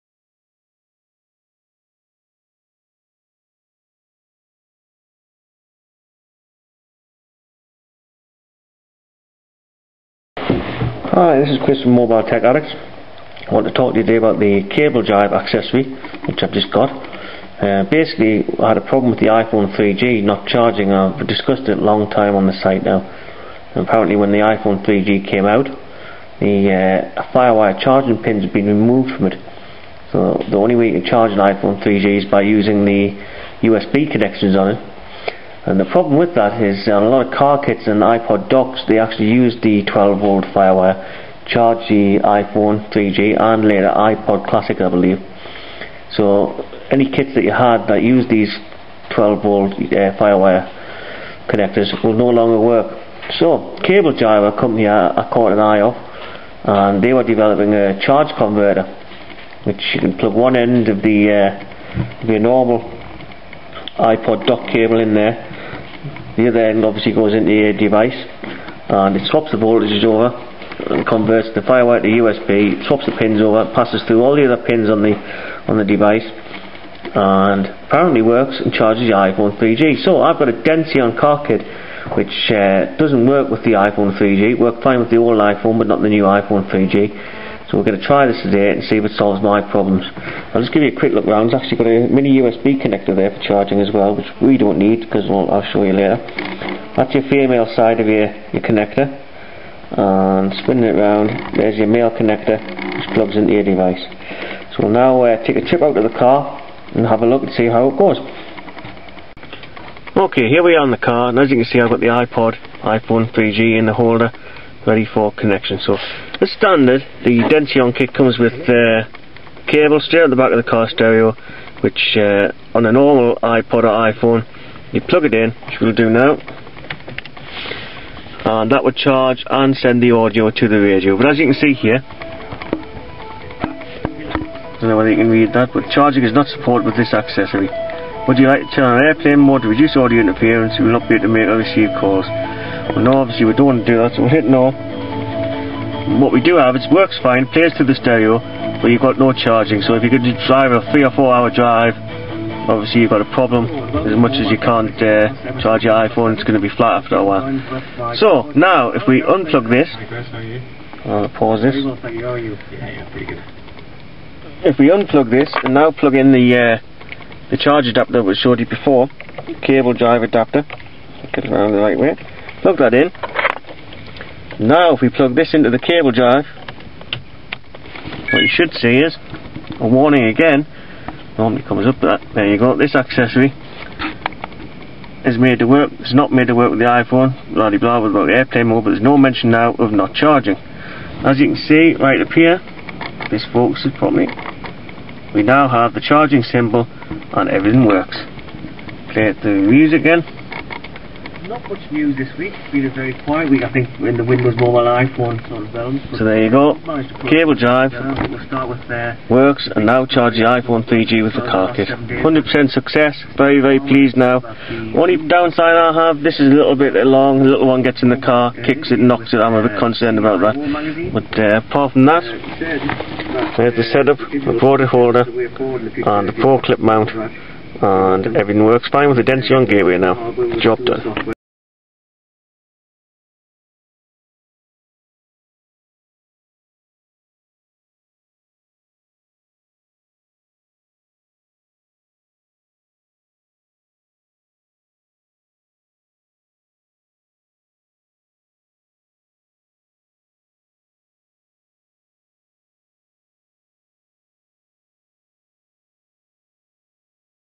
Testing the Cable Jive iPhone 3G charge converter
iPhone 3G does not work with some docks and car kits as it has no Firewire charging pins, this converts the firewire chargeing to USB charging and lets you use previously non compatible accessories for charging.